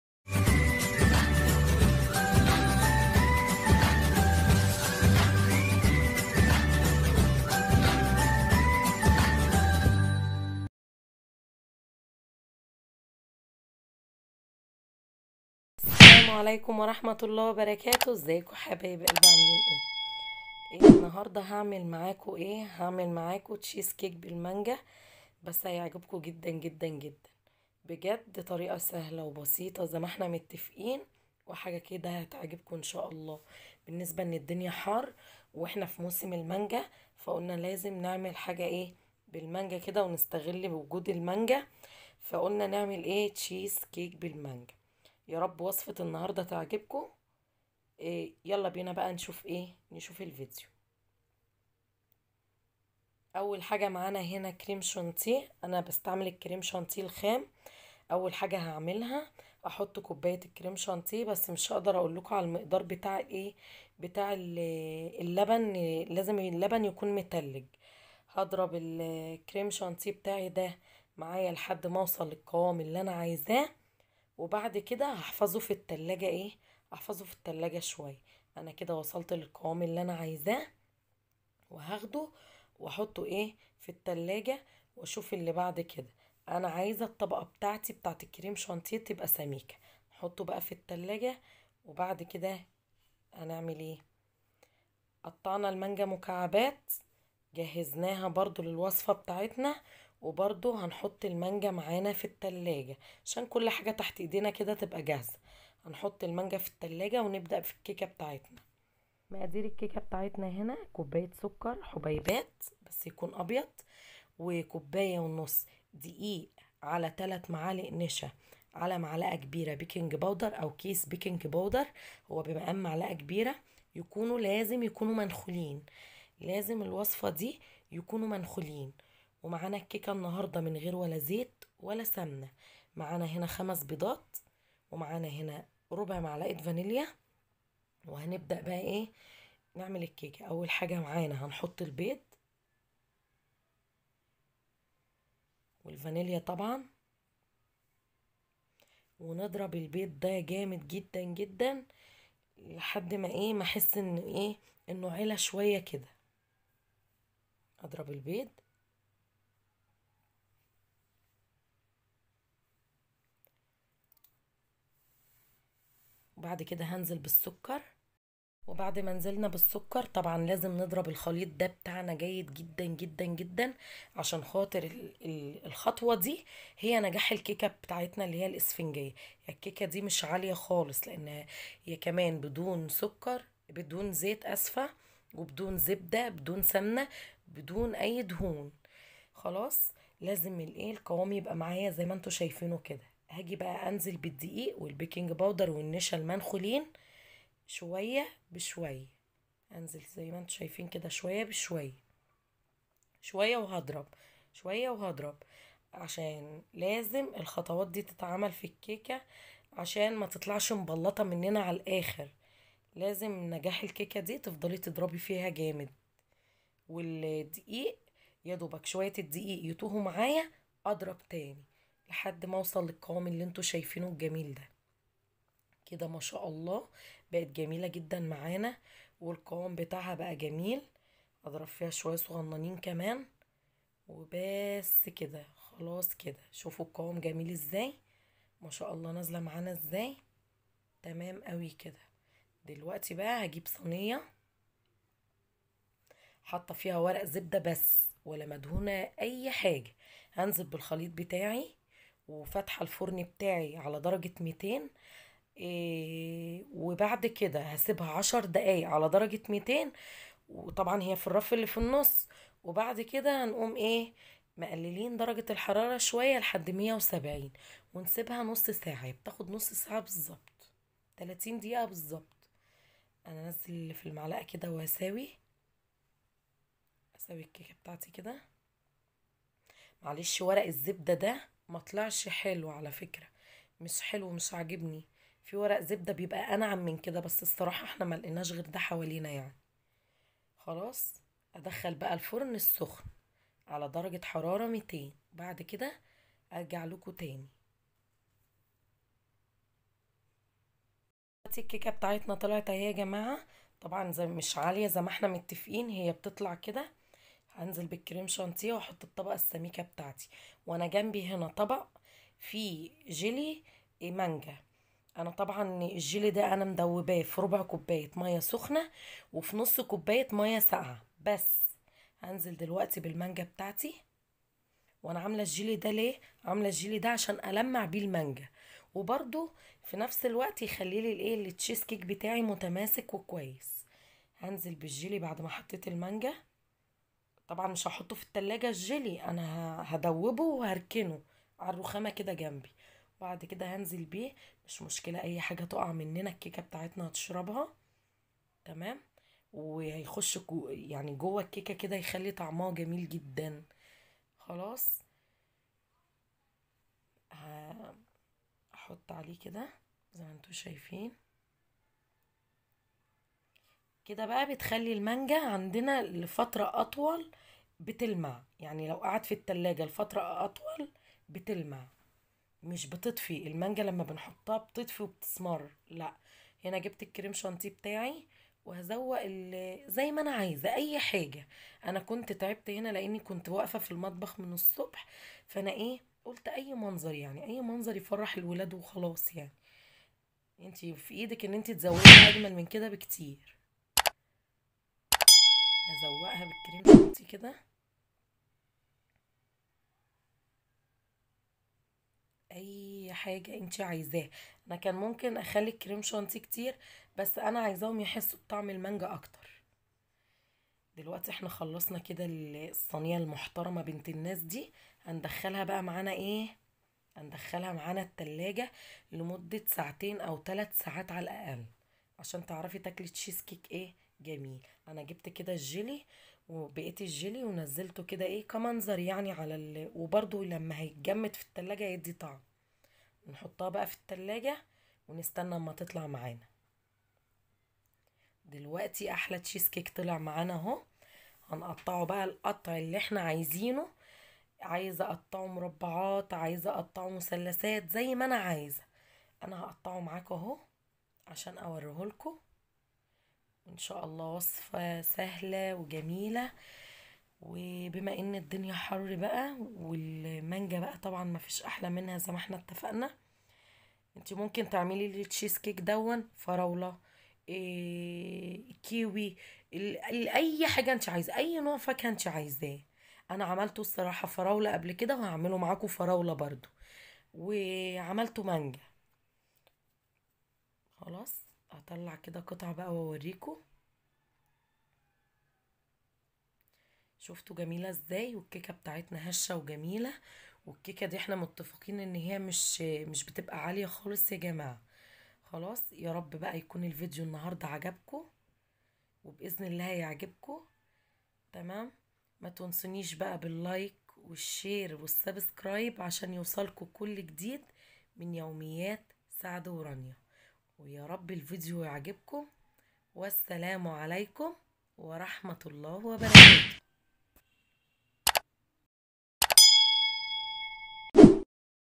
طريقه عمل تشيز كيك بالمانجو طريقه سهله وبسيطه🎉🥰
طريقه عمل تشيز كيك بالمانجو طريقه سهله وبسيطة مانجا طبيعيه طازه فريش قطع مانجو تشيز كيك المانجو
Faceboo